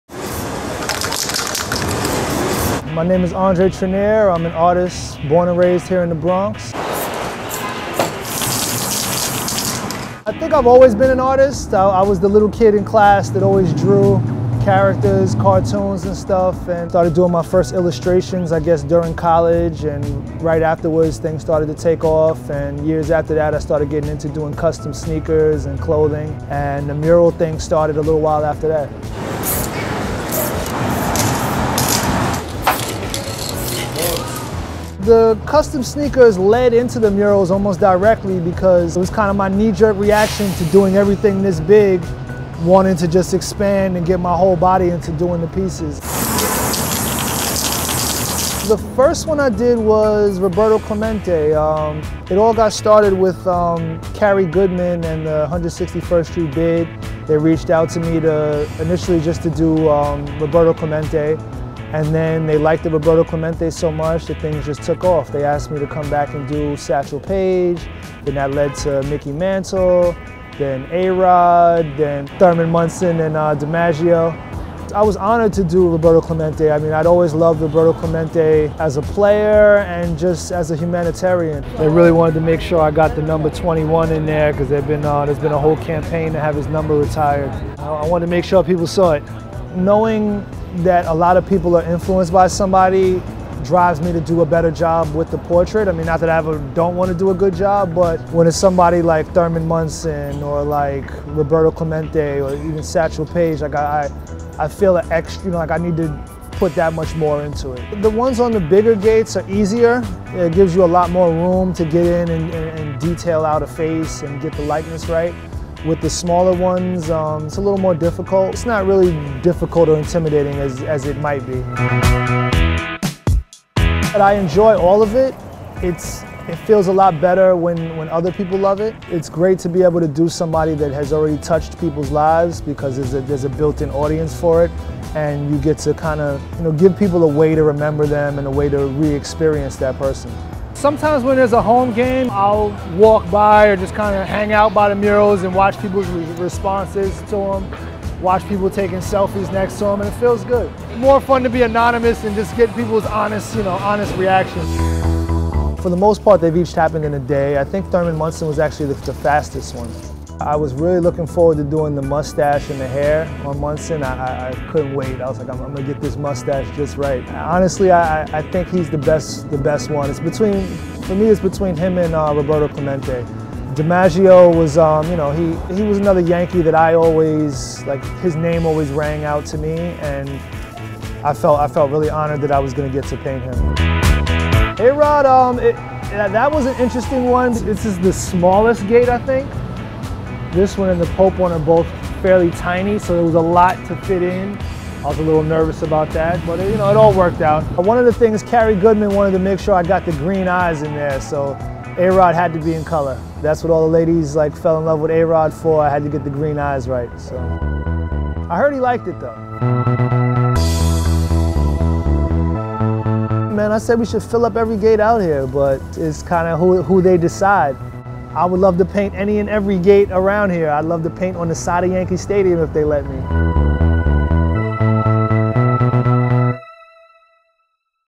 New York Yankees Mural Artist, Andre Trenier
Meet the man behind some of the Yankees murals along River Ave. in the Bronx, Andre Trenier.